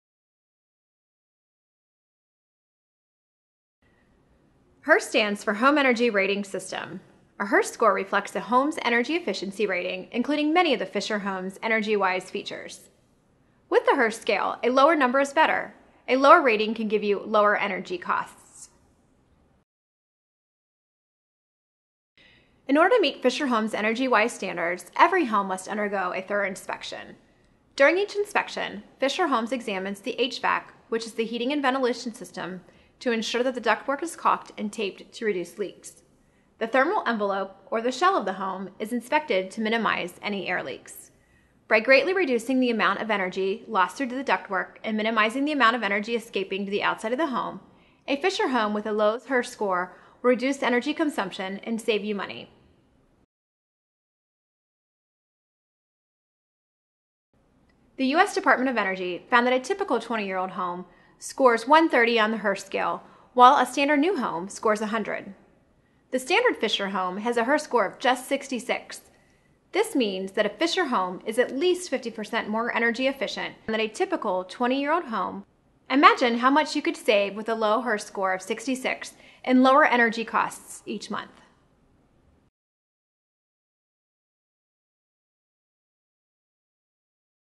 Understanding a HERS Score by Fischer Homes
Learn more about what a HERS Score is and how it can impact your monthly heating and cooling costs.